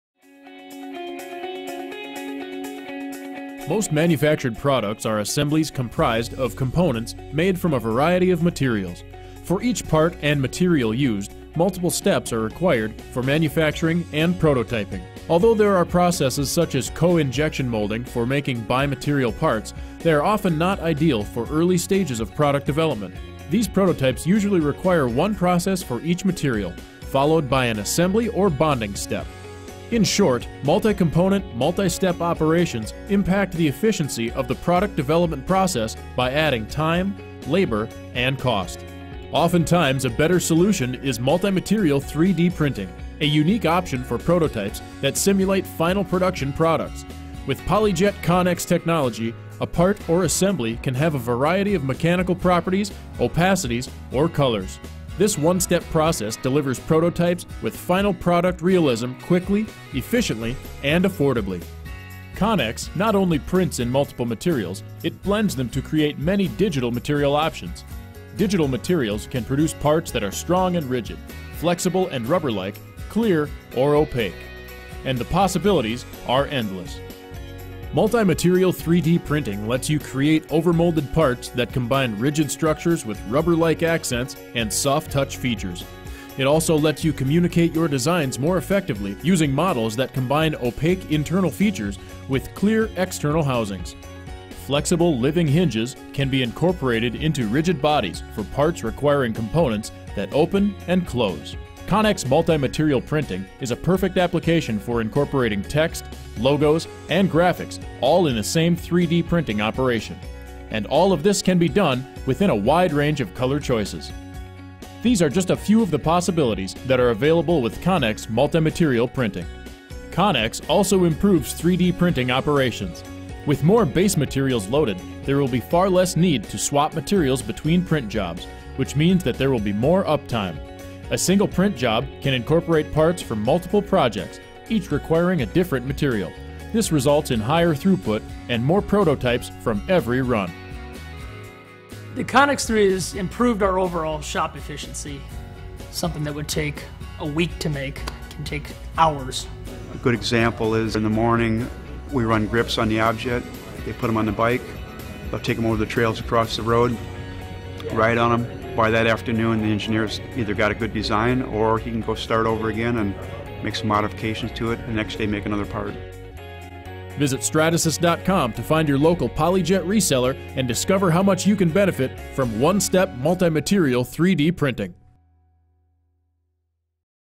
Stampa 3D multimateriale | Stratasys PolyJet | Applicazioni
Le stampanti 3D Stratasys con tecnologia PolyJet permettono di stampare  in 3D con diversi materiali per realizzare prototipi di prodotti che riproducono perfettamente quello che sarà l'oggetto reale. Materiali di stampa rigidi, gommosi, opachi, trasparenti, per ottenere modelli 3D monocromi o a colori.